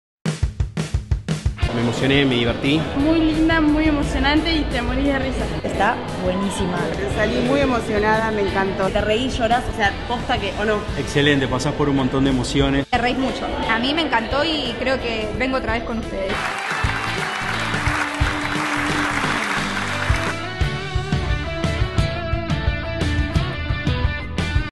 100 METROS CUADRADOS - TEATRO MULTITABARIS COMAFI
Sinopsis:
Se vende un departamento de 100 metros cuadrados en el mejor barrio de la ciudad y para Sara es el piso ideal, ya que reúne todas los requisitos que ella busca para mudarse: Amplitud, luz, una hermosa vista y un precio inmejorable. Solo un pequeño inconveniente separa a Sara de la posibilidad de mudarse apenas pague el departamento: Tendrá que esperar que Lola, la anciana dueña del departamento: muera. Teniendo en cuenta la avanzada edad de la señora, sus tres bypass y su adicción al tabaco y al alcohol, Sara calcula estar viviendo en su nuevo piso en menos de dos años. Pero la vida siempre nos depara sorpresas y entre constantes risas, carcajadas y alguna que otra lágrima, Sara y Lola, (Florencia Bertotti y María Valenzuela) nos contarán en esta desopilante comedia, como la vida se encarga, día a día, de demostrarnos que nada es lo que parece ser, y que nadie puede asegurar los hechos y sucesos que nos acontecerán. Decía John Lennon: ”que la vida es eso que te va sucediendo mientras uno se encuentra ocupado en hacer otros planes con ella”. Y esta magnífica comedia se encargará, escena a escena y acto a acto, de demostrarlo